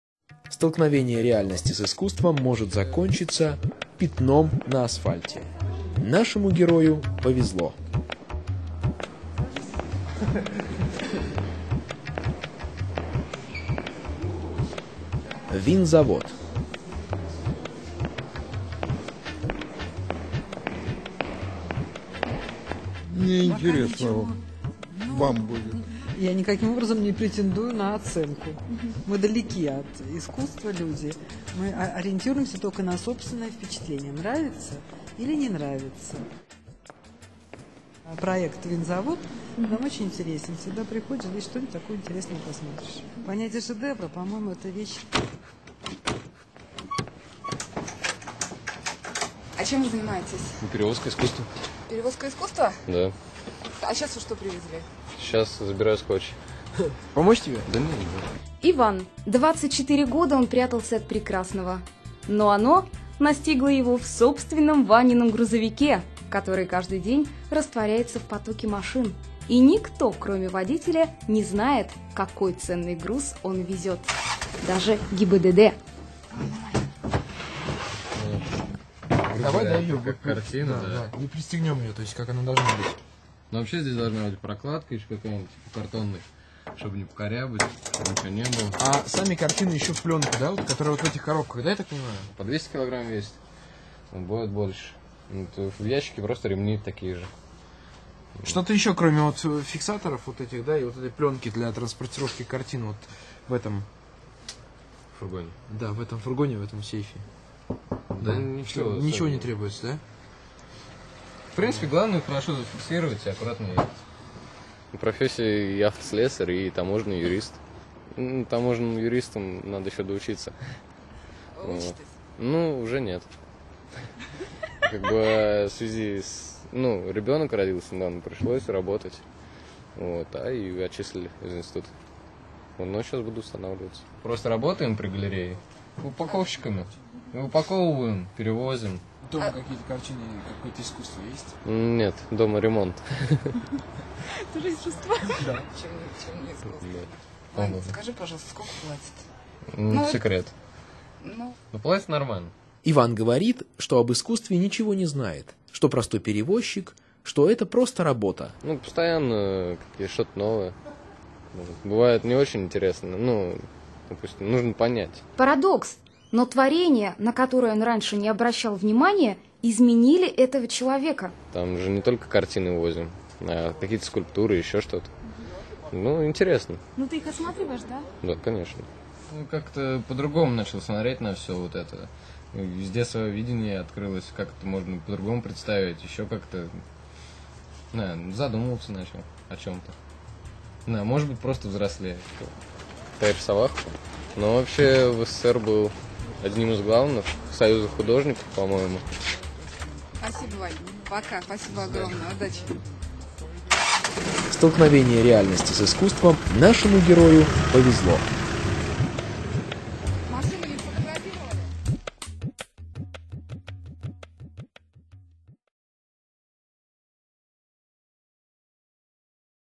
realartmmover13
Упаковщик №13 на Московском Винзаводе. Он перевозит искусство как другие перевозят пластиковые окна или кур.